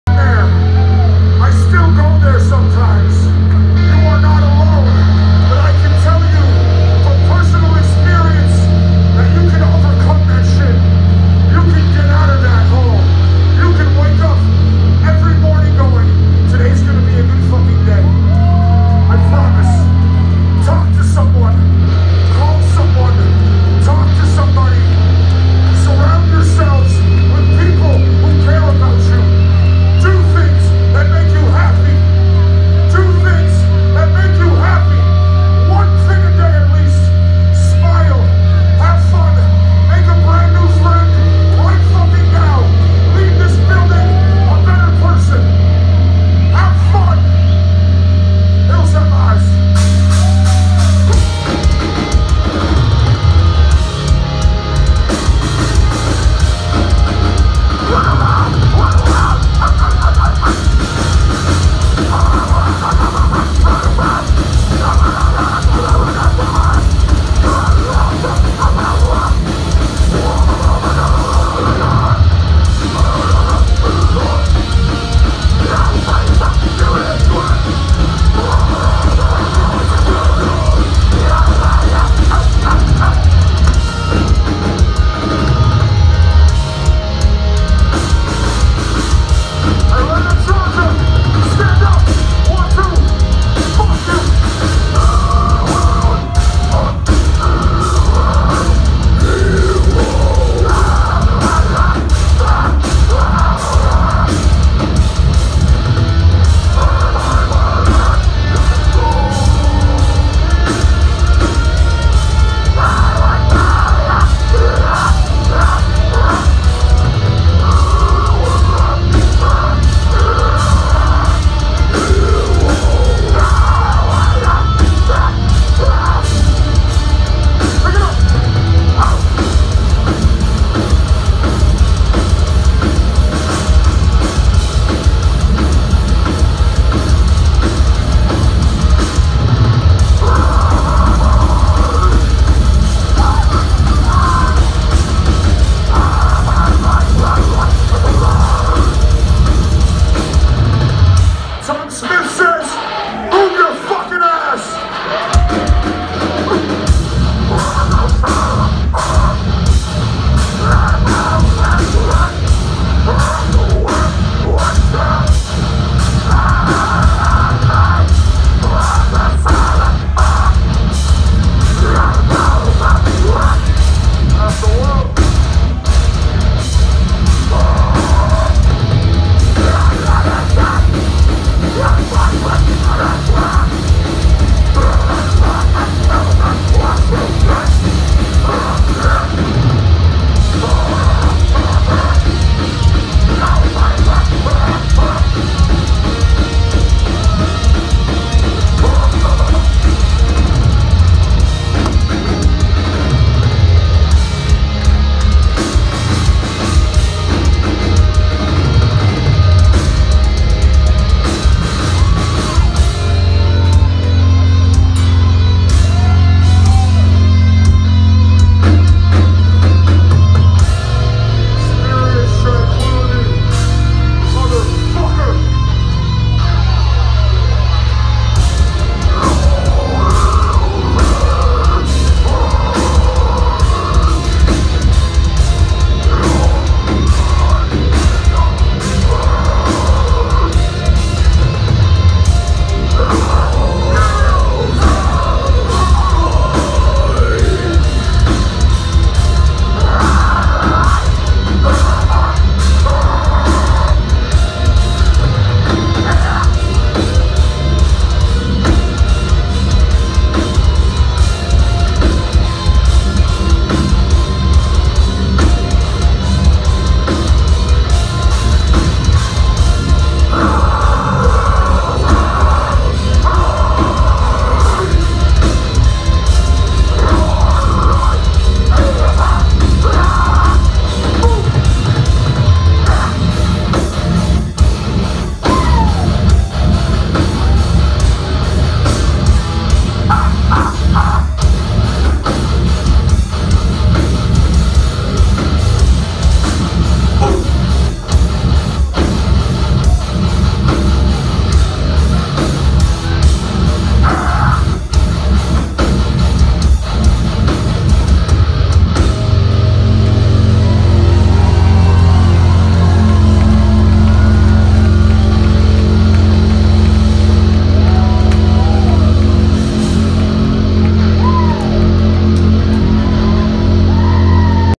The Acacia Strain - @The masquerade (Purgatory) - 3.7.2020
The Acacia Strain playing in Purgatory @ the masquerade on March 7th 2020.